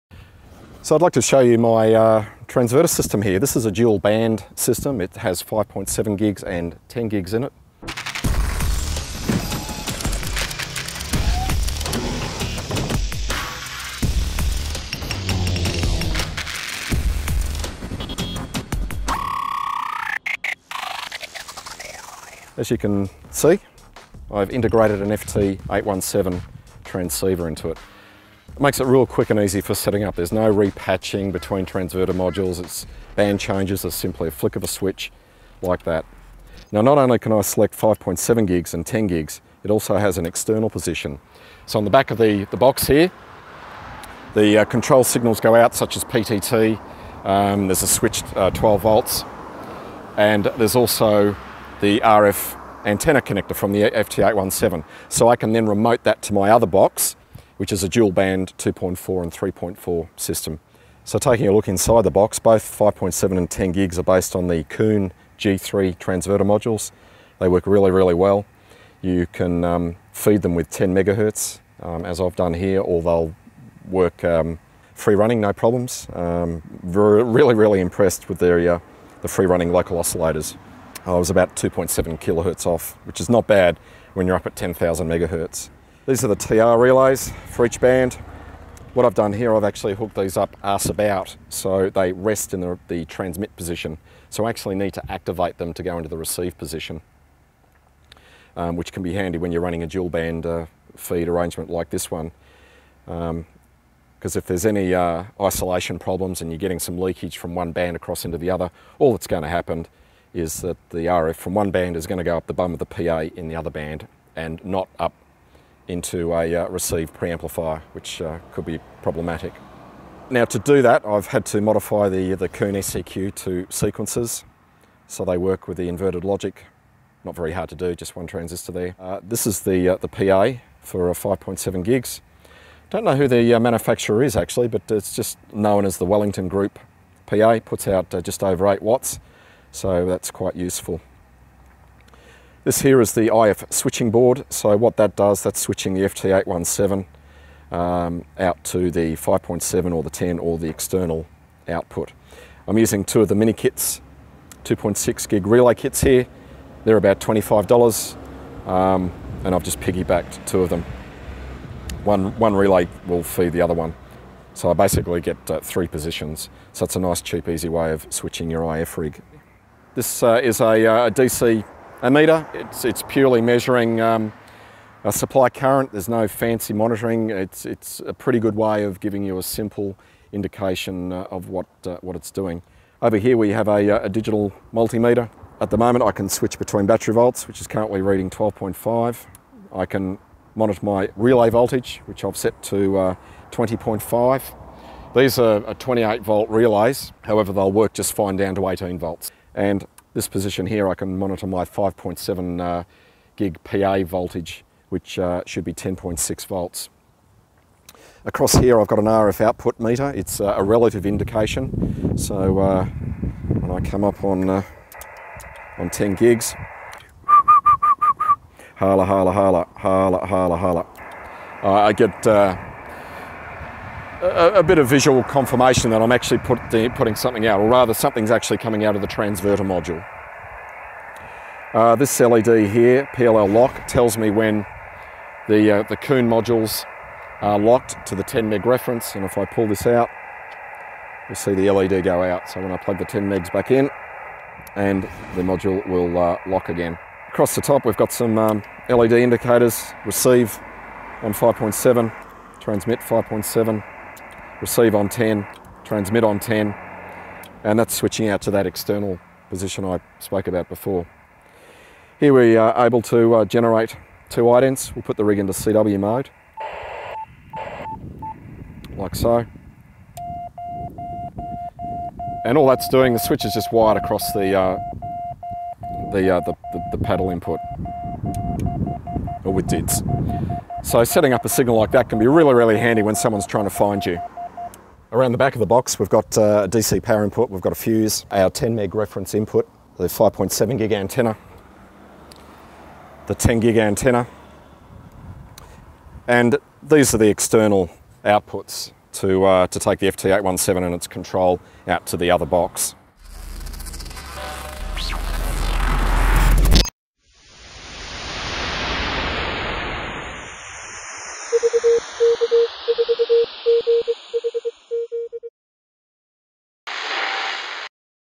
Dual-band 5.7/10 GHz microwave Transverter
Take a look at VK4GHZ's dual band transverter system.
Based on Kuhne G3 modules, an integrated FT-817 IF transceiver makes band switching instant.